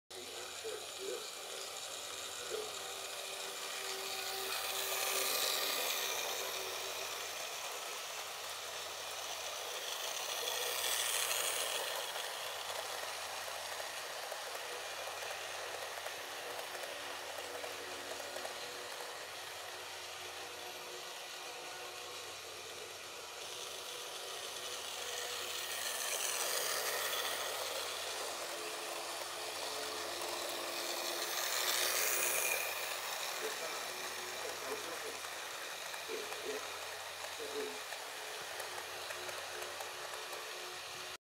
Zwei Mal Roco TT V100
Roco Press V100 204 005 mit Kesselzug und 202 781 mit gemischten Güterzug drehen ihre Runden